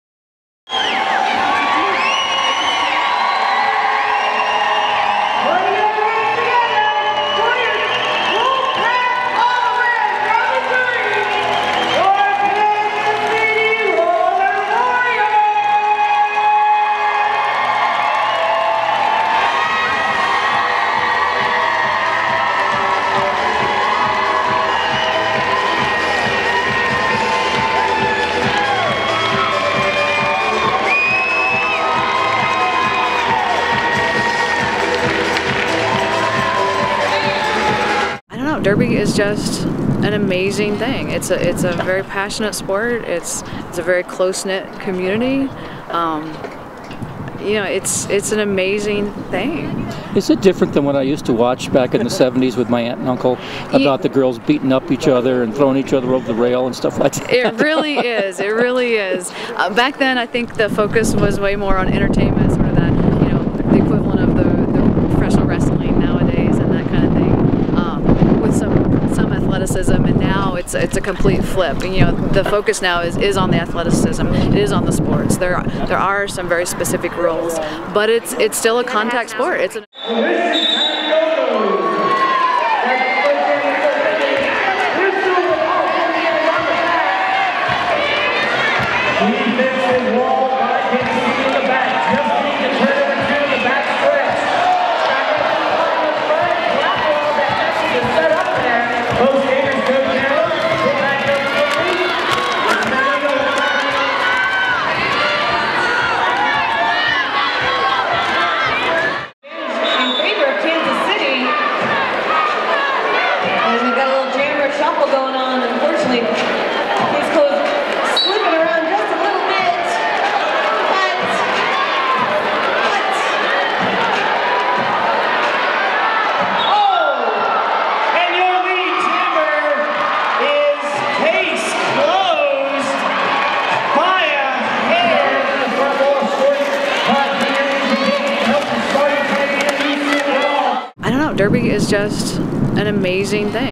Roller Derby Championships
The 2011 WFTDA Roller Derby Championships were held November 11-13th at the 1ST Bank Center in Broomfield, CO. Video by Cliff Grassmick